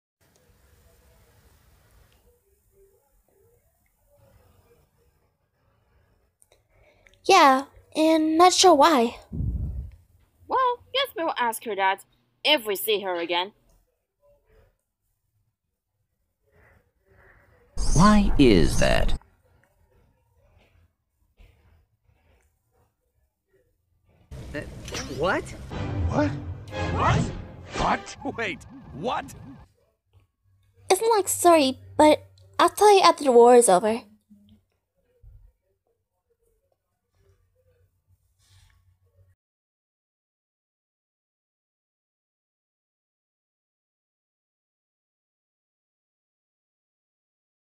Lines for Thecrystopilisempire
For Sonja, I did some lines as my Sonja's adventure series OC, Kelly. I hope you like it!